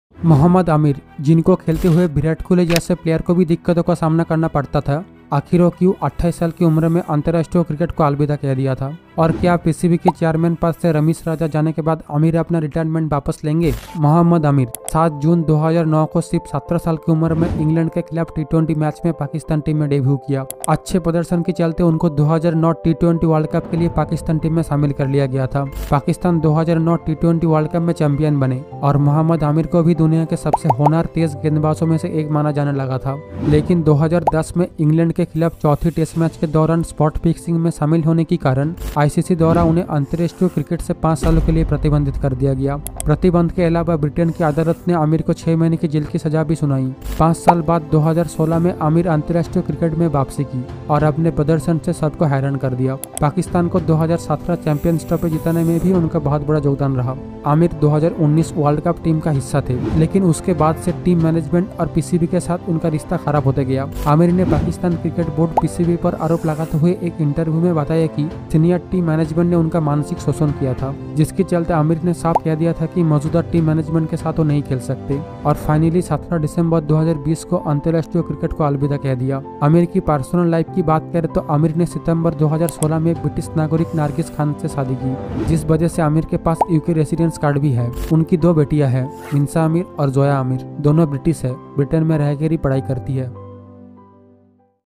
क्या मोहम्मद आमिर अपने रिटायरमेंट वापस लेंगे?Will Mohammad Amir take back his retirement?
Mohammad Amir Cricket Analysis🔻

Mohammad Amir is a Pakistani cricketer who is considered one of the most talented fast bowlers in the world. He made his international debut for Pakistan in 2009 at the age of 18 and quickly established himself as a key player for the national team. Amir's left-arm fast bowling style, combined with his ability to swing the ball both ways, has made him a formidable opponent for batsmen. He has represented Pakistan in all three formats of the game, including Test matches, One Day Internationals and Twenty20 Internationals. Despite a brief hiatus from the sport due to a spot-fixing scandal in 2010, Amir made a successful comeback and continues to be a vital member of the Pakistani cricket team. In this video, we take a look at some of Amir's greatest moments on the field and his impact on the game of cricket.

If any person has objections to the clips used in this video, then he can contact the channel manager.